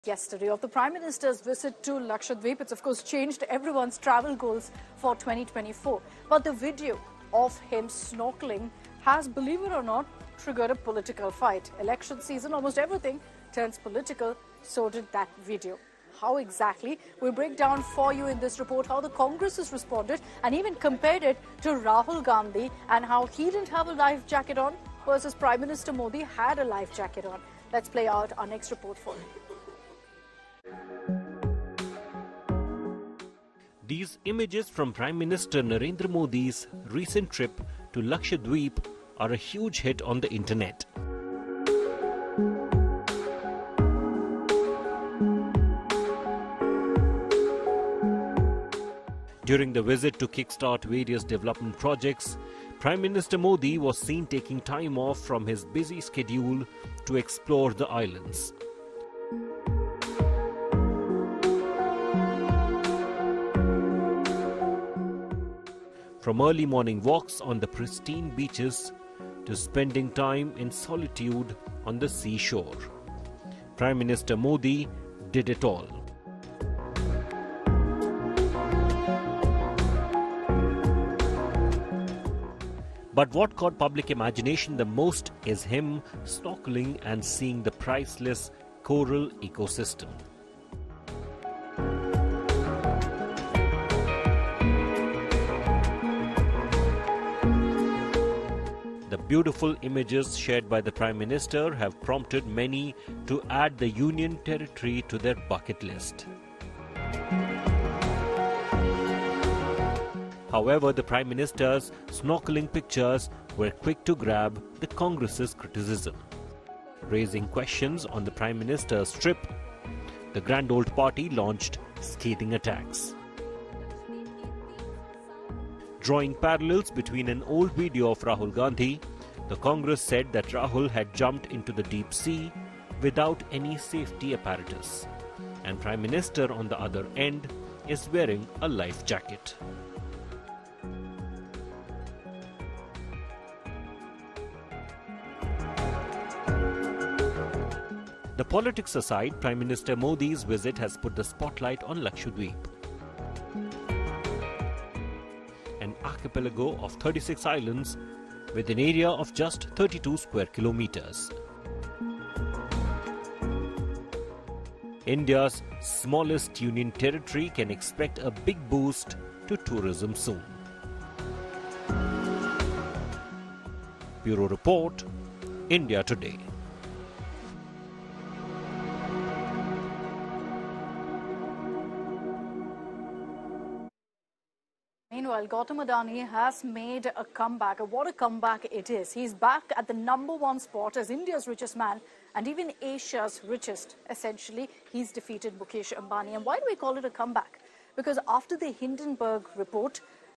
Prime Minister's Snorkelling In Lakshadweep Fuels Political Debate
Concerning the Prime Minister's recent visit to Lakshadweep, the main source of online buzz was a video of him snorkeling. The Congress party has used this as occasion to contrast him with Rahul Gandhi, who they pointed out did not wear a life jacket when he went into the sea. While the political controversy unfolds, the actual event has placed Lakshadweep, India's smallest Union Territory, under the touristic limelight. The Prime Minister was also there to initiate various development projects. Comprehensive analysis of this political debate and its potential impacts on tourism are covered in this report.